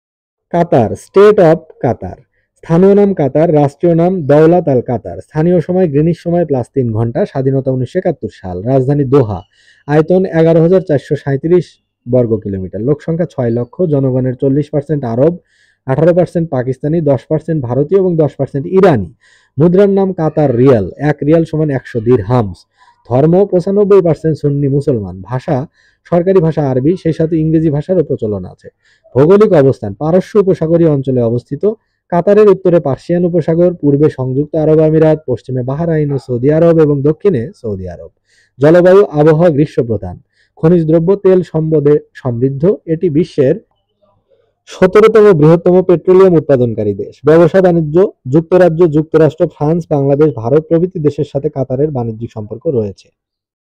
কাতার দেশ পরিচিতি
কাতার দেশ পরিচিতি। কাতার দেশ সম্পর্কে কিছু সাধারণ তথ্য। কাতারের রাজধানী দোহা।